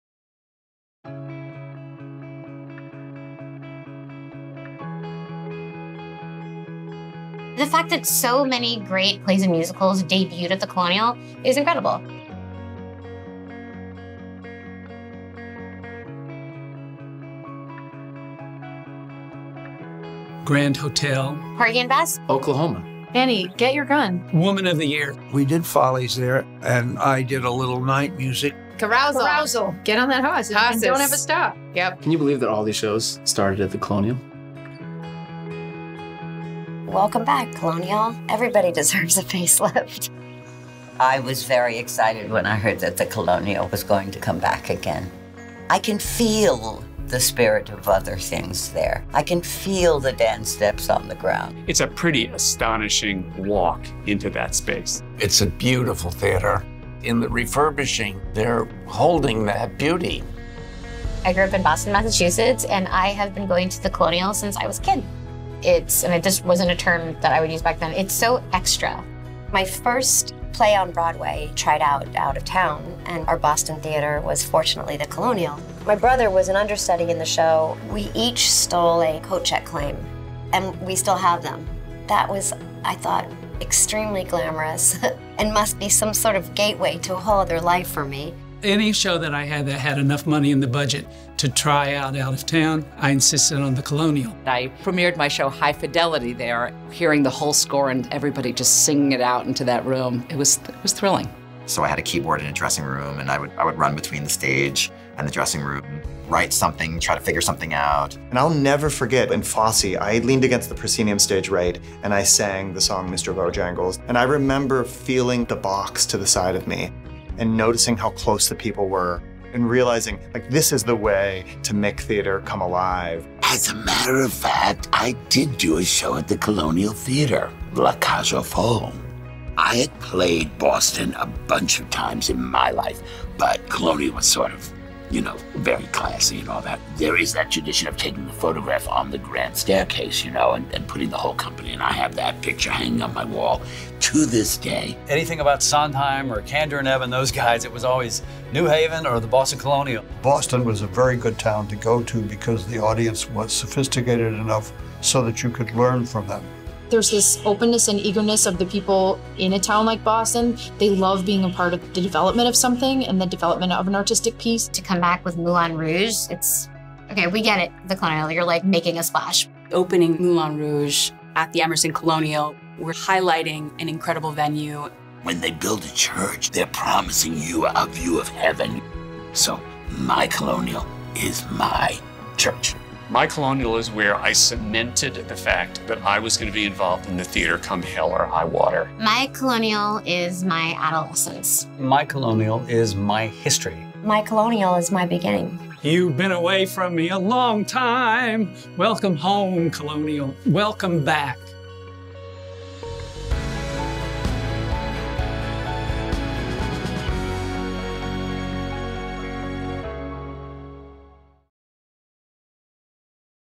myColonial
So many of us have a Colonial story. Together they are MyColonial.
To commemorate the re-opening of one of the most historic theatres in the country, a group of luminaries including Harvey Fierstein, Mindy Kaling, Sarah Jessica Parker, Chita Rivera, Tommy Tune and more gathered to share their stories about Boston's crown jewel.

Share your stories with us - here, and across social media. Use the hashtag myColonial so we're sure to see them.

Credits:
"Parade on Boylston Street" photo courtesy of the Boston Public Library, Leslie Jones Collection.

Various black and white photography of Colonial Theatre by Friedman-Abeles © The New York Public Library.

Boylston St, 1902 (Ben Hur marquee) and Horse & Buggy Photography: Boston Pictorial Archive, Boston Public Library.

"Woman of the Year" Grand Staircase Company Photo and "My One and Only" Grand Staircase Company Photo, both courtesy of Jon B. Platt Foundation.

Hal Prince and "Follies" Company Grand Staircase photo by Bill Yoscary.

"La Cage aux Folles" Grand Staircase photo by Jon B. Platt.

Photos of the refurbished Emerson Colonial Theatre by Patrick Farrell.